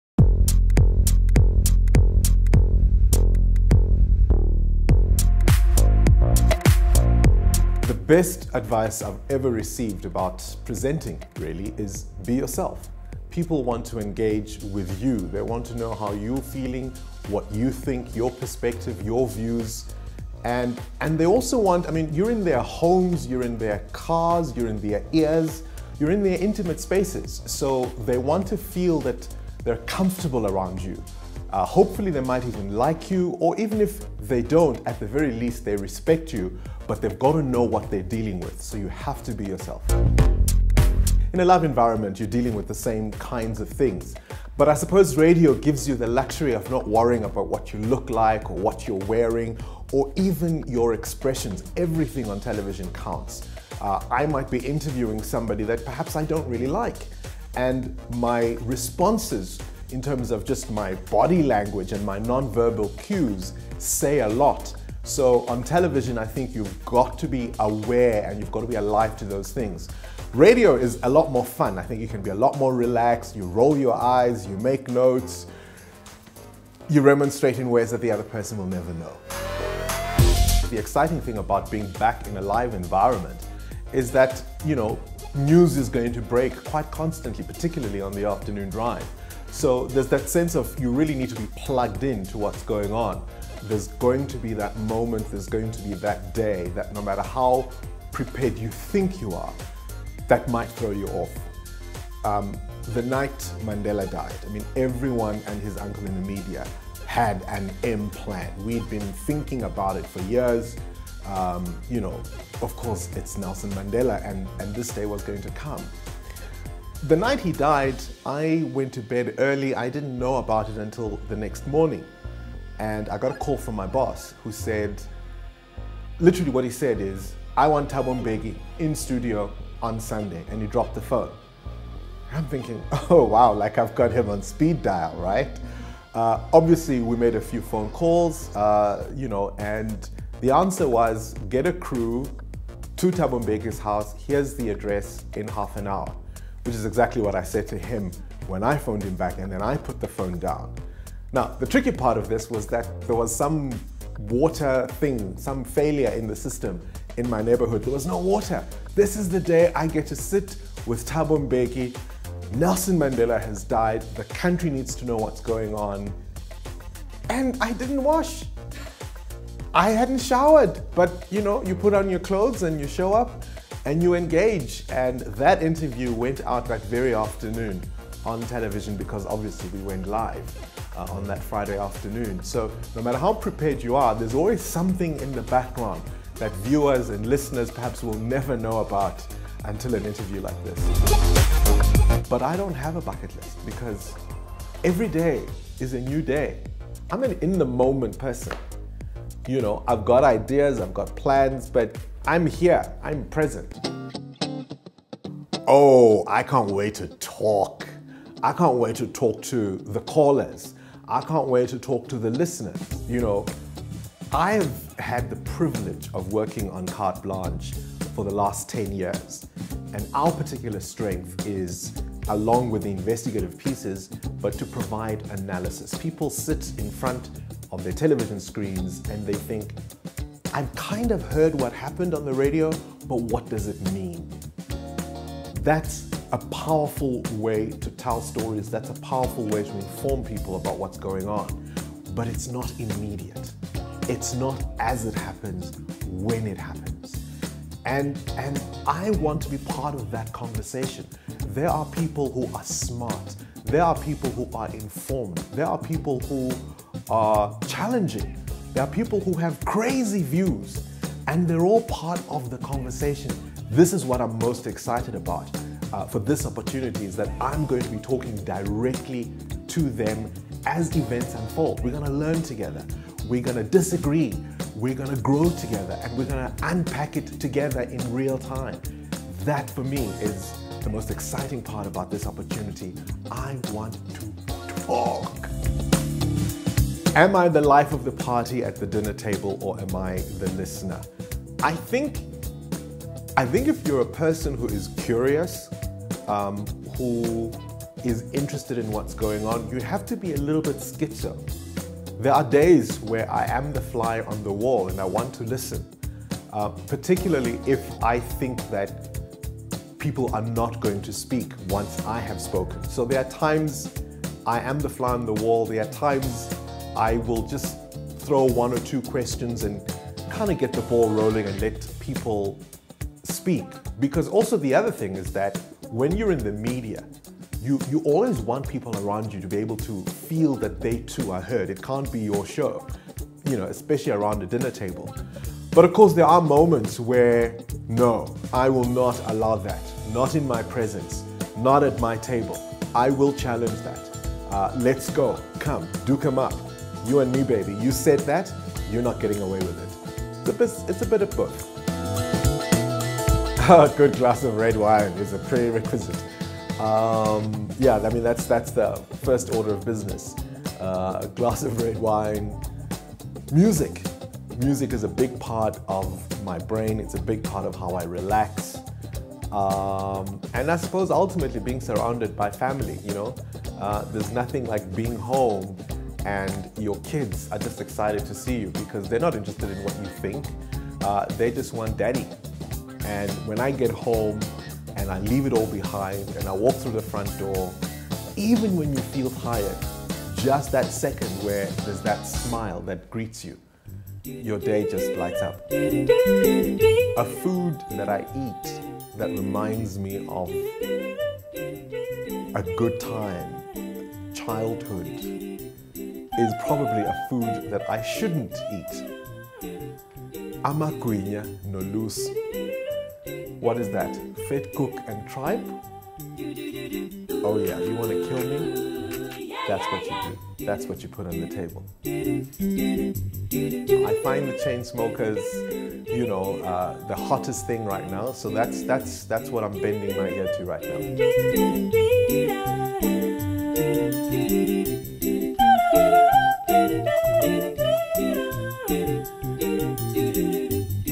Timeout with Bongani Bingwa
Find out more about 702's new Afternoon Drive host, Bongani Bingwa.